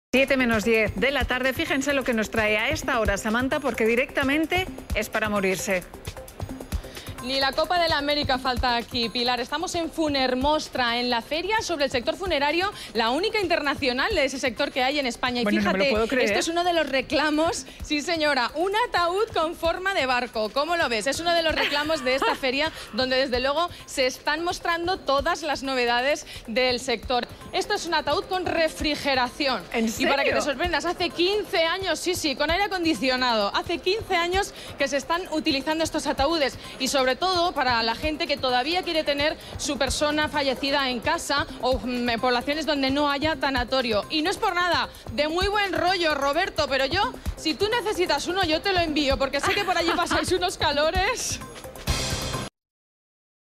Ataúd con refrigeración
En la feria del sector funerario de Valencia, Funermostra, ha sido presentado un ataúd con refrigeración "para la gente que todavía quiere tener su persona fallecida en casa" (10/05/2007)